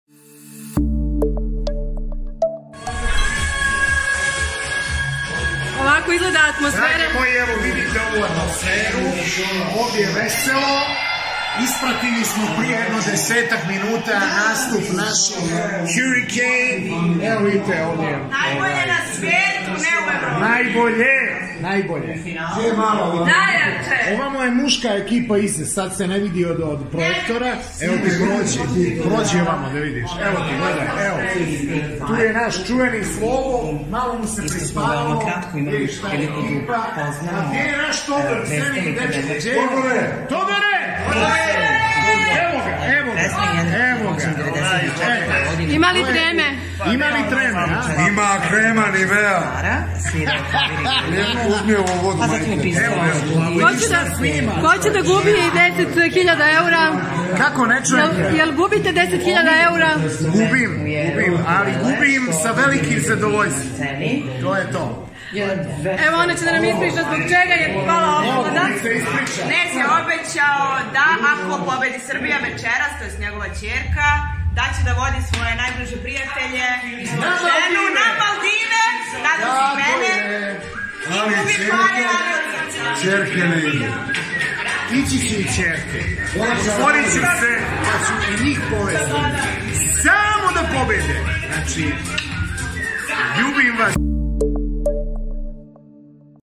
KNEZ SE KLADIO U 10.000 EVRA, a onda su nastele SCENE ZA PAMĆENJE u njegovom domu tokom glasanja
Naša ekipa se nalazila u domu Ksenije Knežević, članice grupe “Hurricane”, koja večeras predstavlja Srbiju na pesmi "Evrovizije".

Knez, Ksenijin dečko, rođenda sestra, maćeha i prijatelji navijaju svim snagama, atmosfera je sjajna a pala je i jedna opklada i to u 10.000 evra.